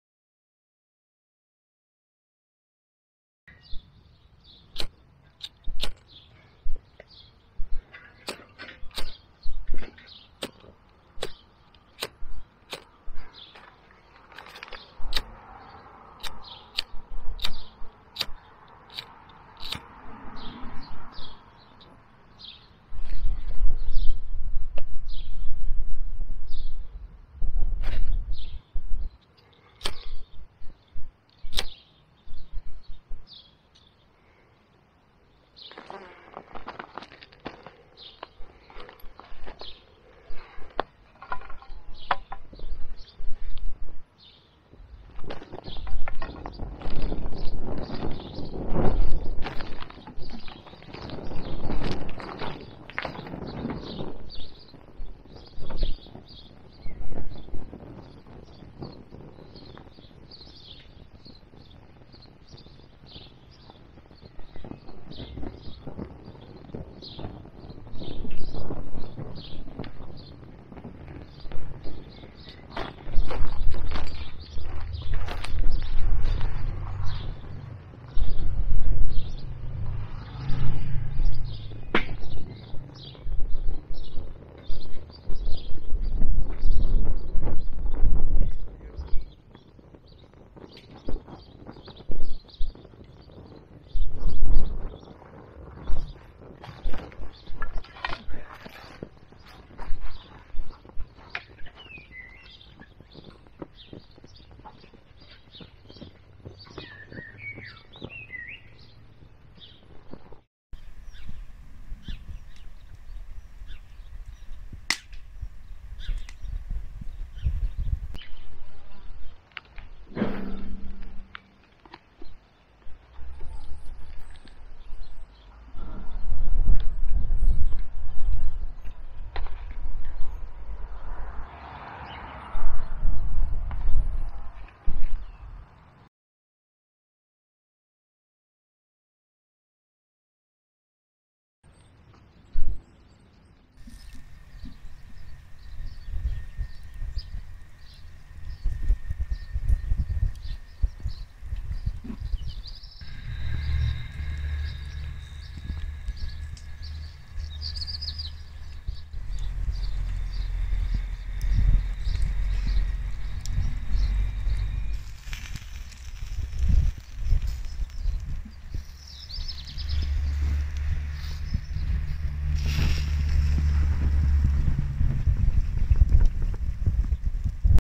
Testvideo | Feuerschale
Testen der neuen Feuerschale und bisschen was ausprobieren mit der Kamera